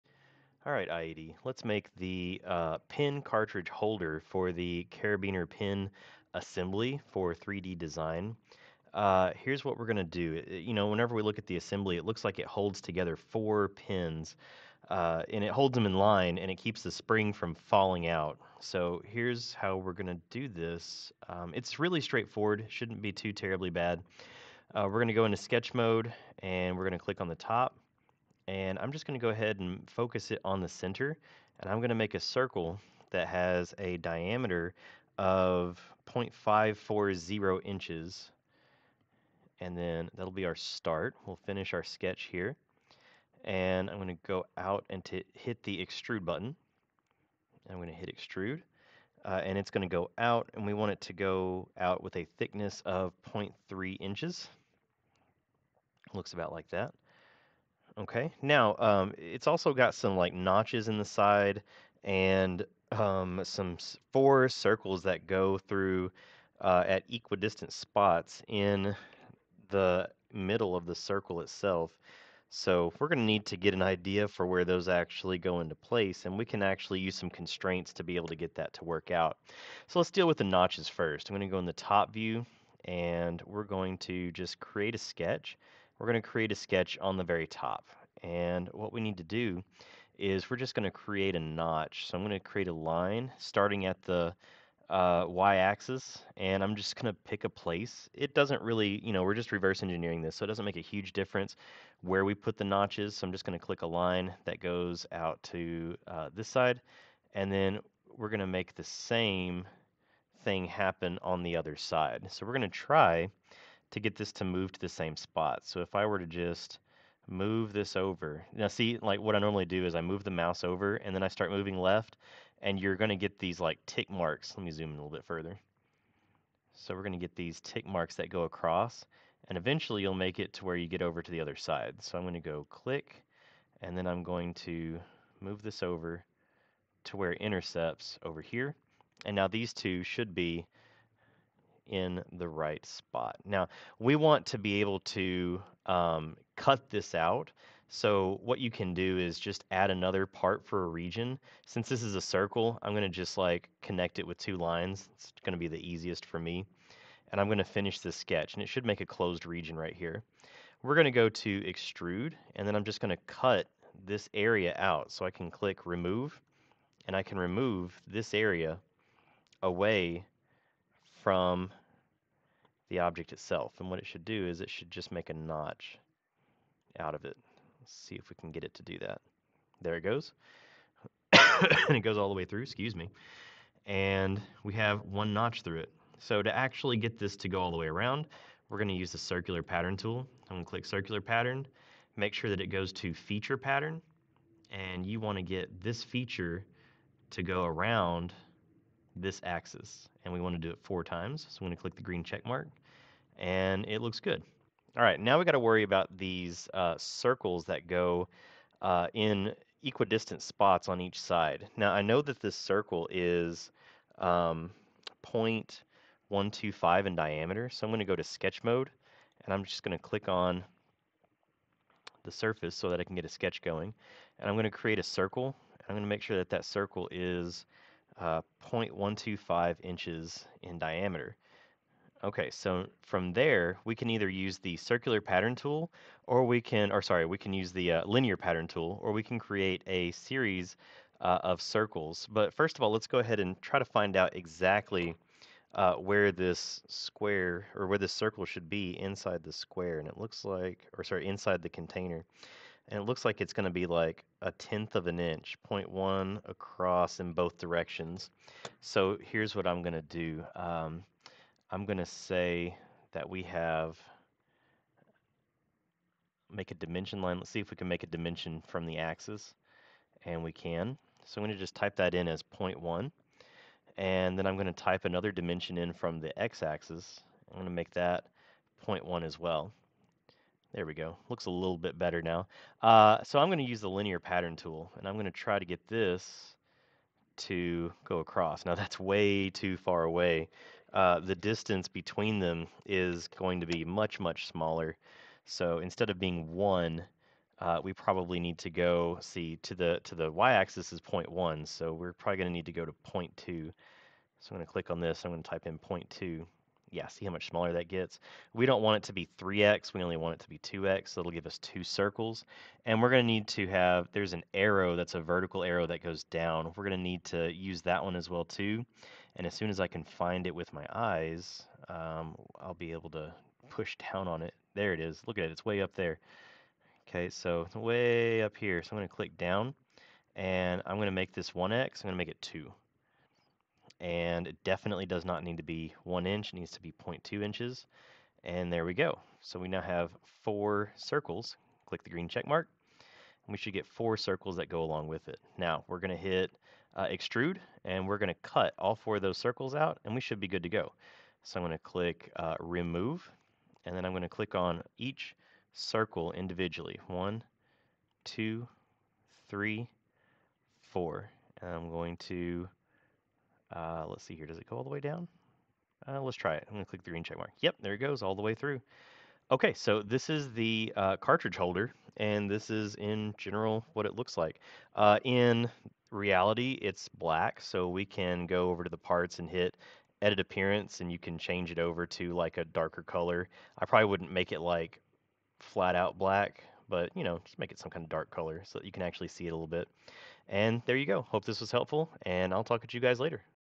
IED 2.2.6 - Top Down or Bottom Up - Part 6 - Carabiner Assembly Cartridge Holder Creation
Creating the Carabiner pen catridge assembly for IED, PLTW 2.2.6 Top down or bottom up.